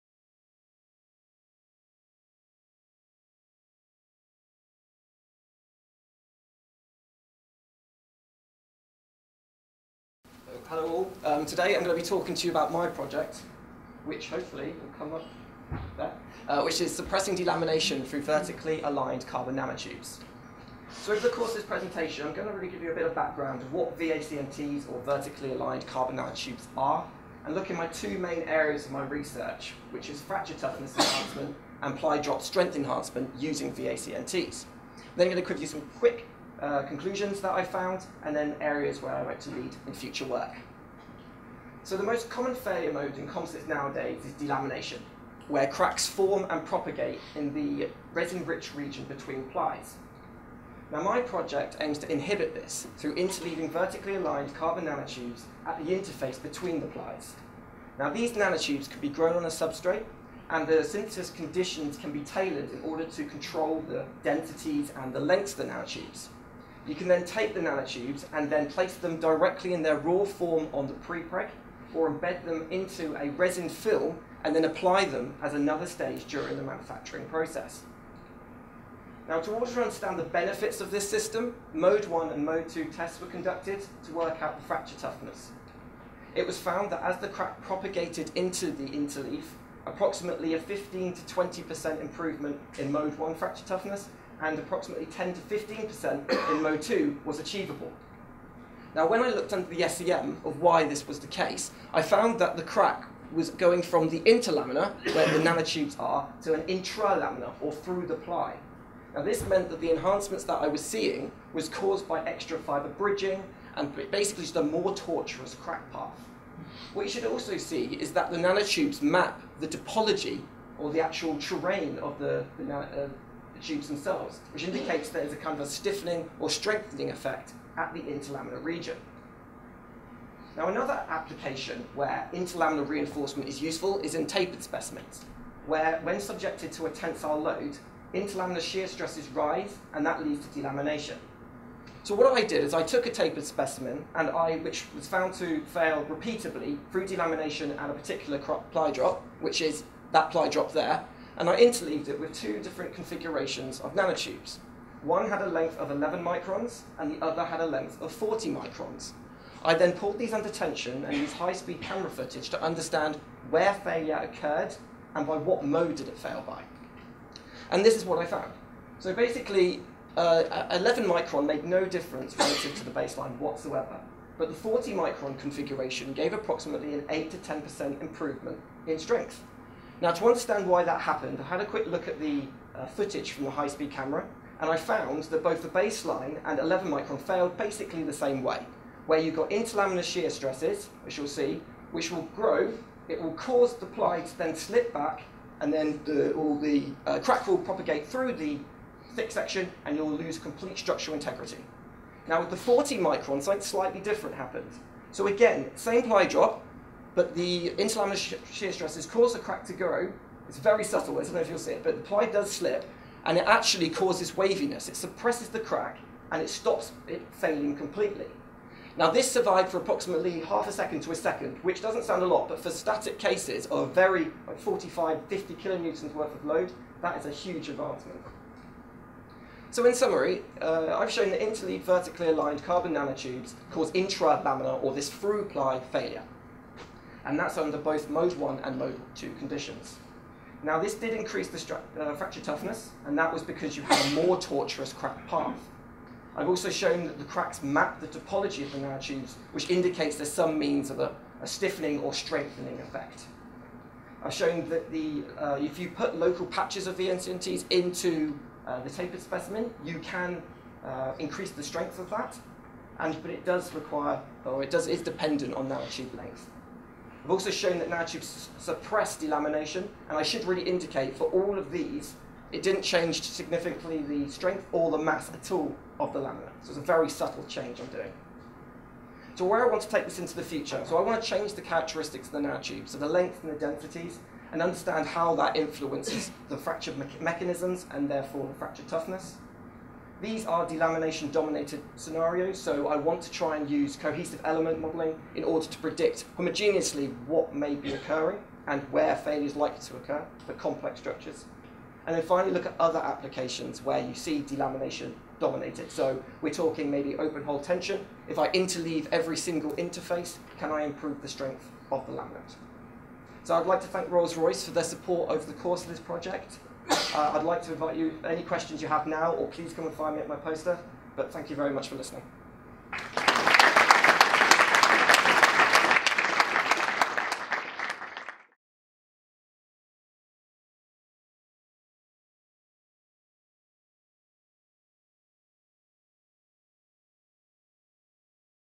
Rob Worboys - Suppressing Composite Delamination through Vertically Aligned Carbon Nanotubes
Presenter: Rob Worboys
Event: Eighth Annual Conference of the EPSRC CDT in Advanced Composites for Innovation and Science (16th April 2019)